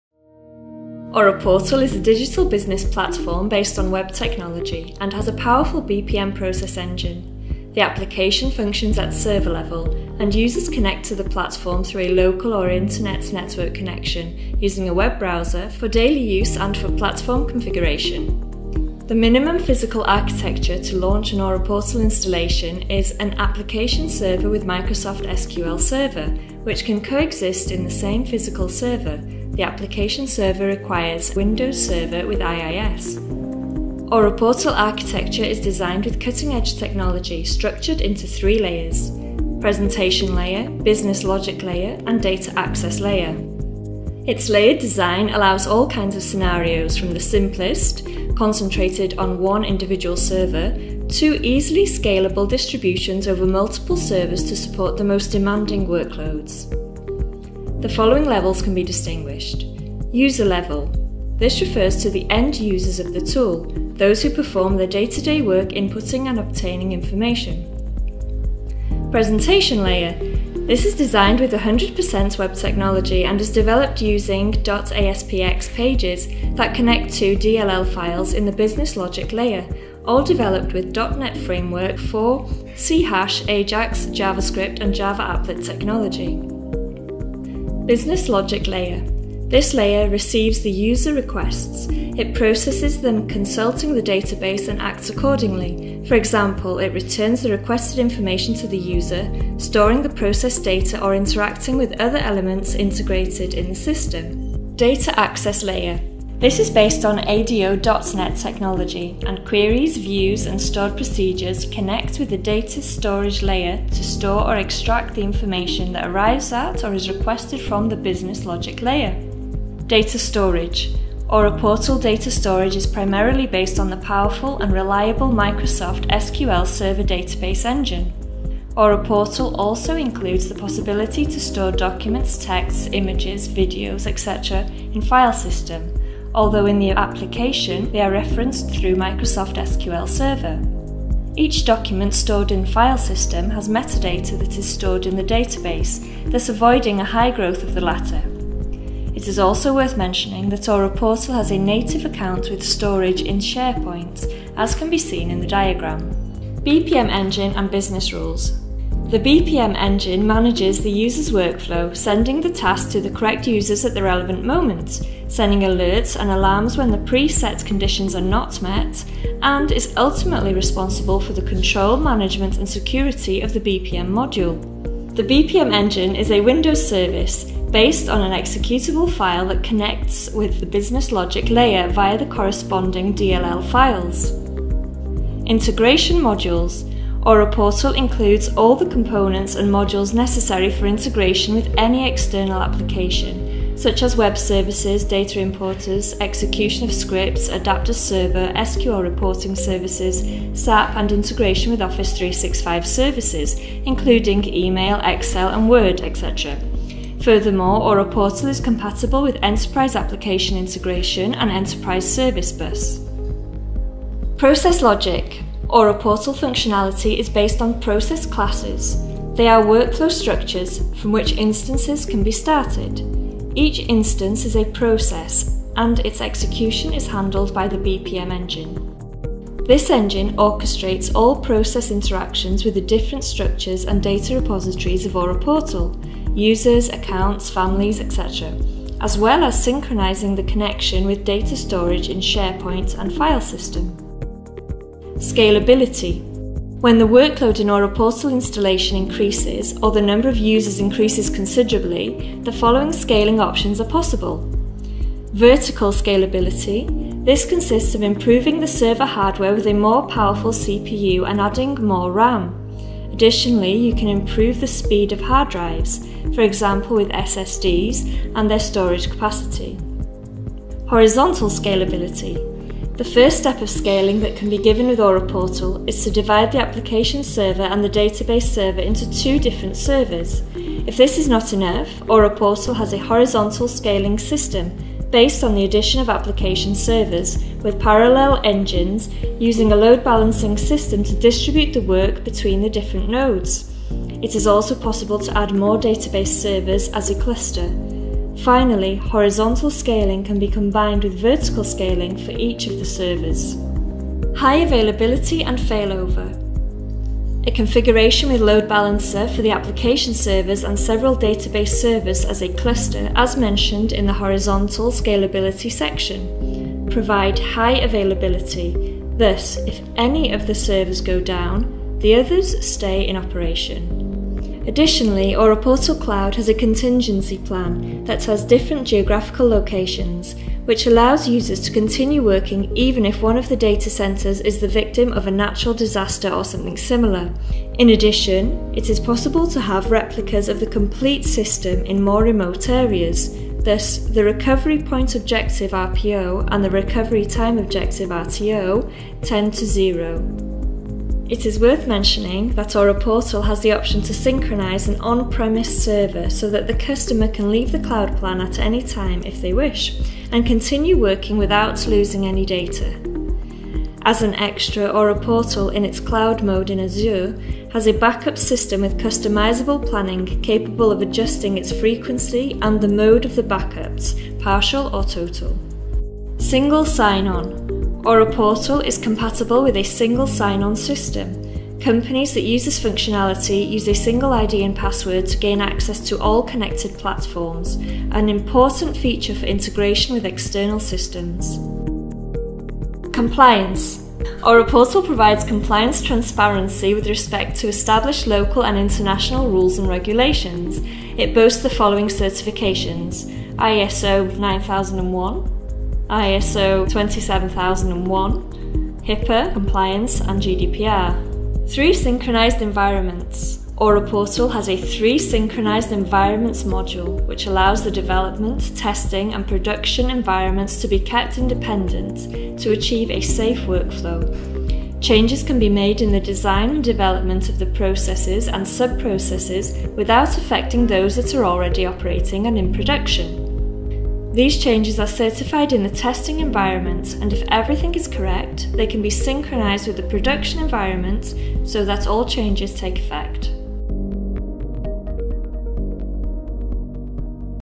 AuraQuantic Architecture (Software & Hardware)
AuraQuantic is a Digital Business Platform based on web technology and has a powerful BPM process engine. The application functions at server level, and users connect to the platform through a local or internet network connection, using a web browser, for daily use and for platform configuration.